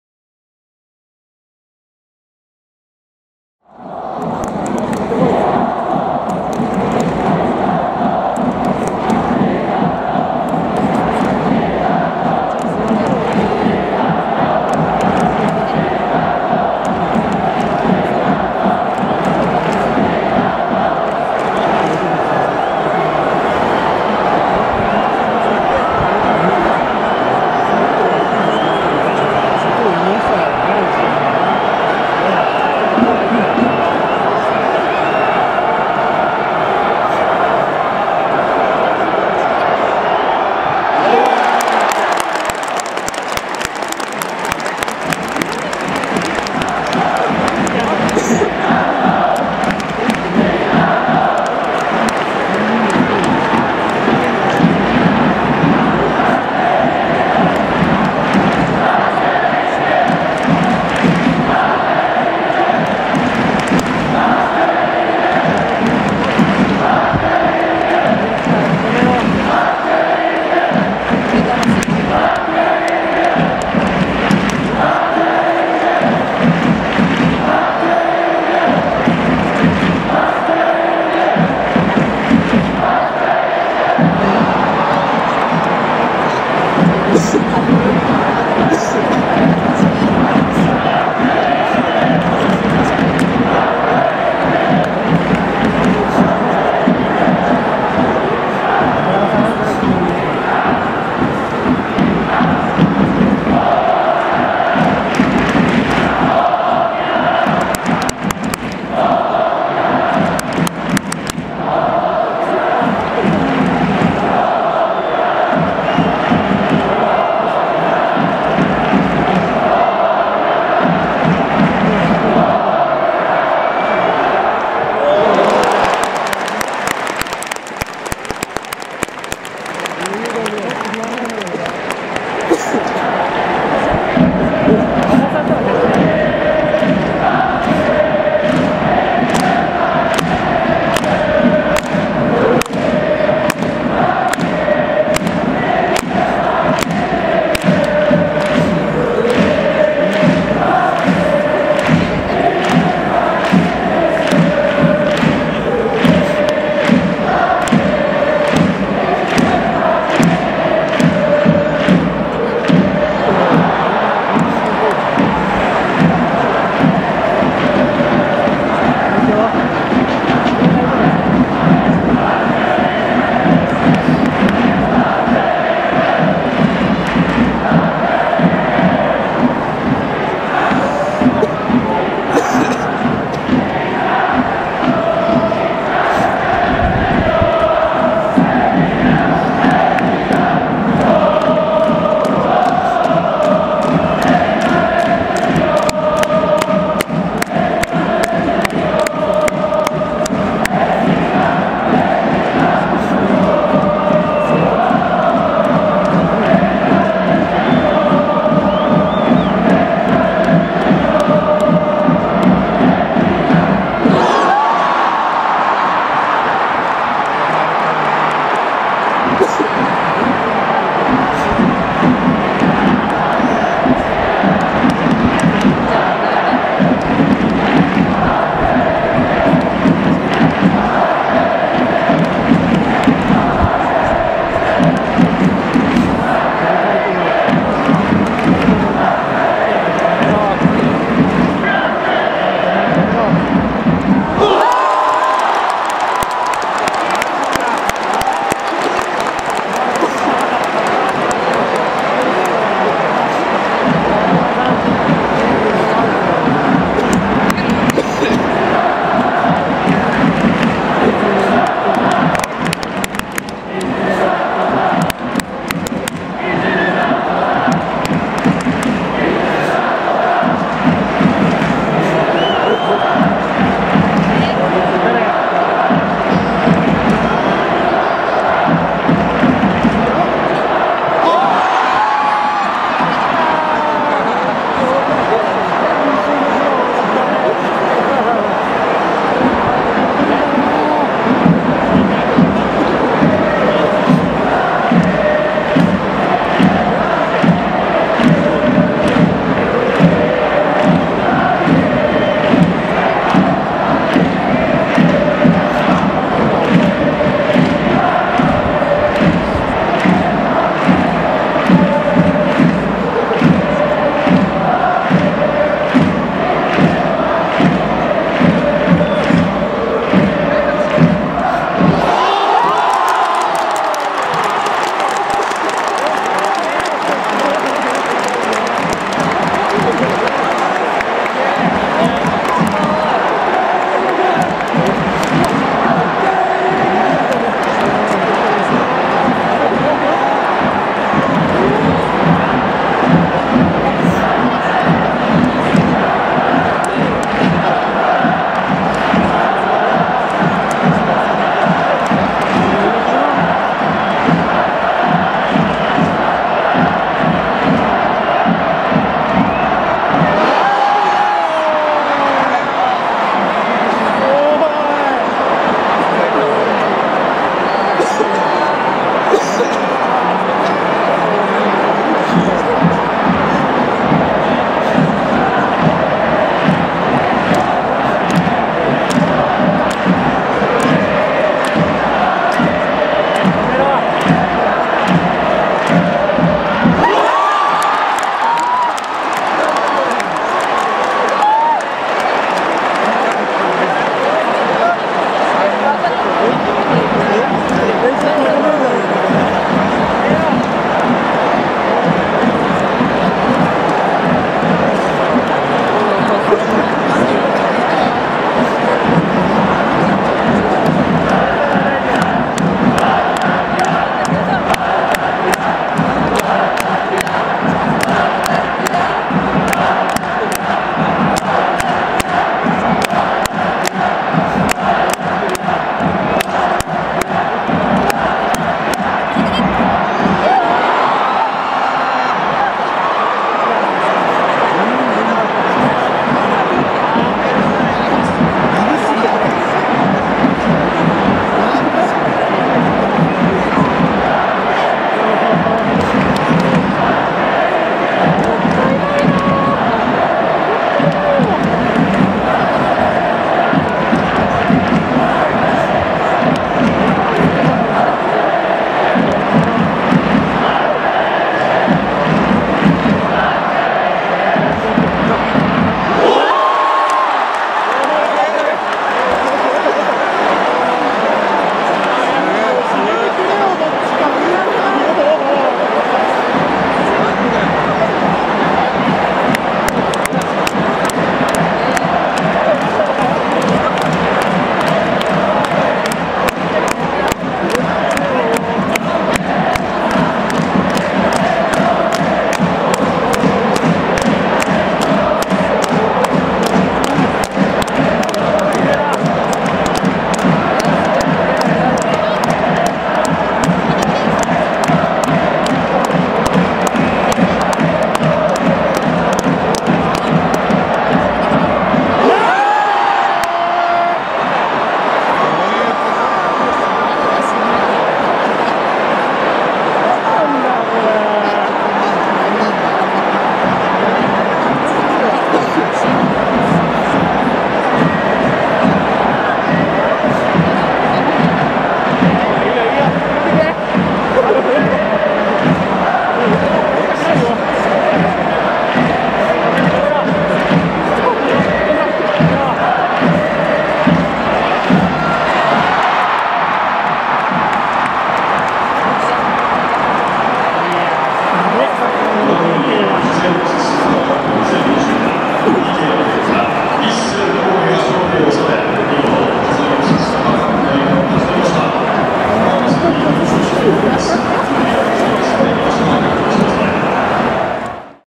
2020.2.8 FUJI XEROX SUPER CUP2020【PK戦】横浜F・マリノス vs ヴィッセル神戸
2019J1リーグチャンピオン vs 第99回天皇杯優勝